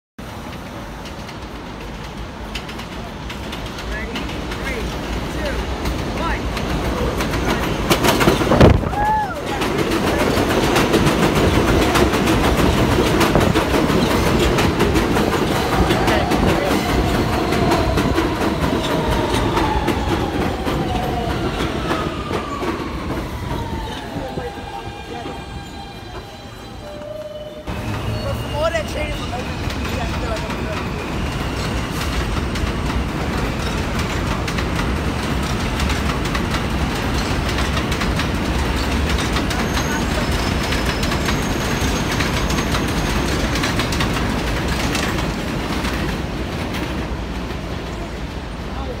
NYC Subway Nostalgia HD 60fps: 2017 Parade of Trains Photo Lineup @ Ocean Parkway (6/17/17)
Annual photo op line @ Ocean Parkway. Enjoy =)